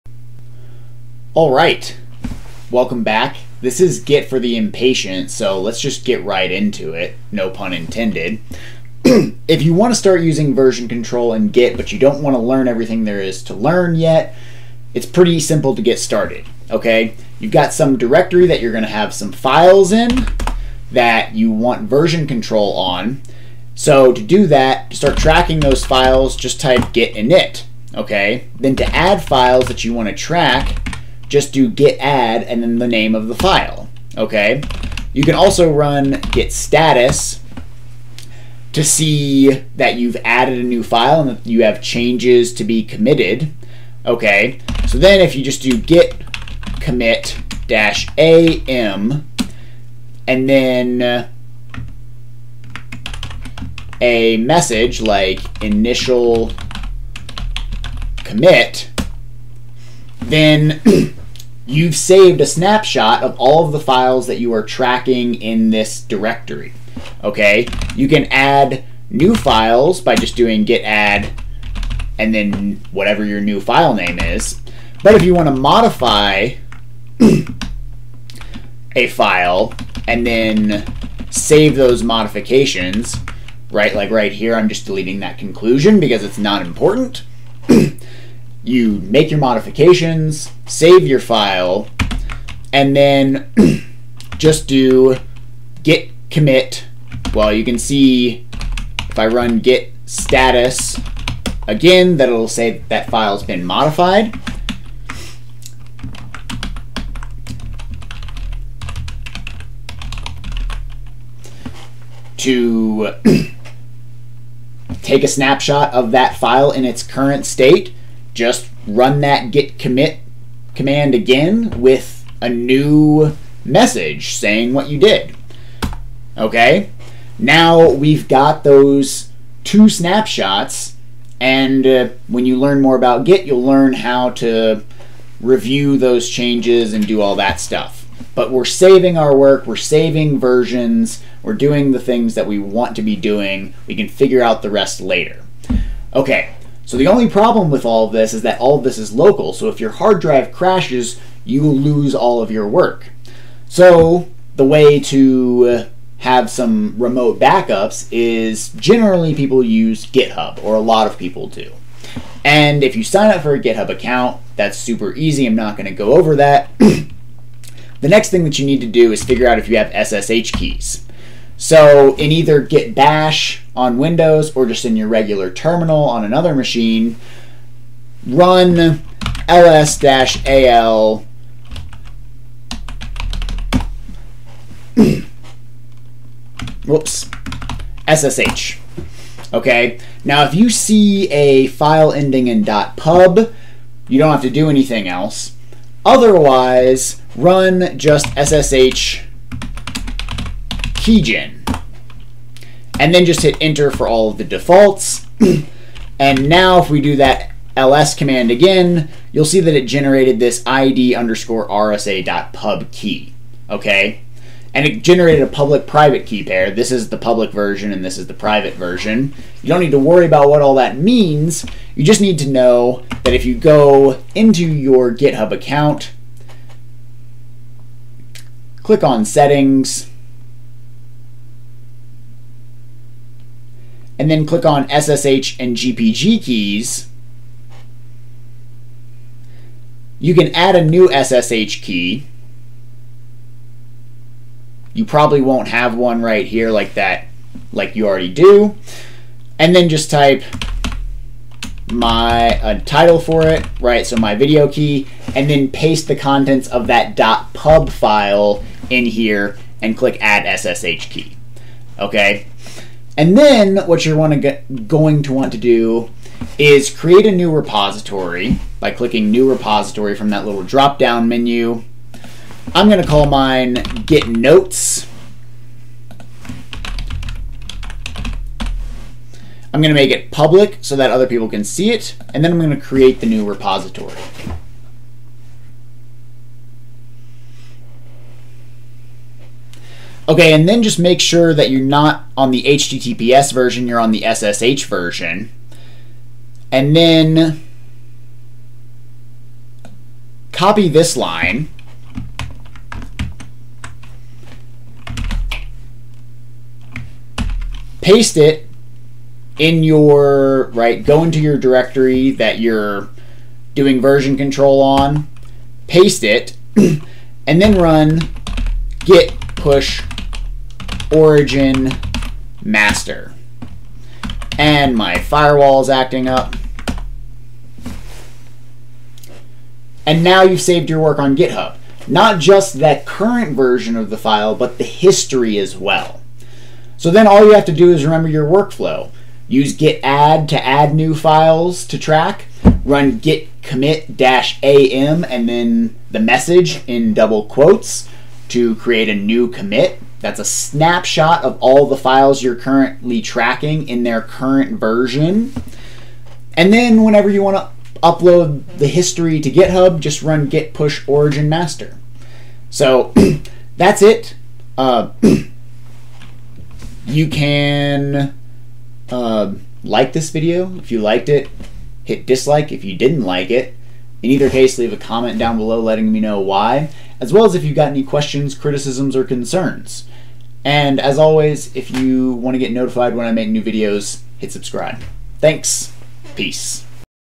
Git for the impatient
How to get/git started with git as fast as possible.

Check out the notes for a future video that goes into git more thoroughly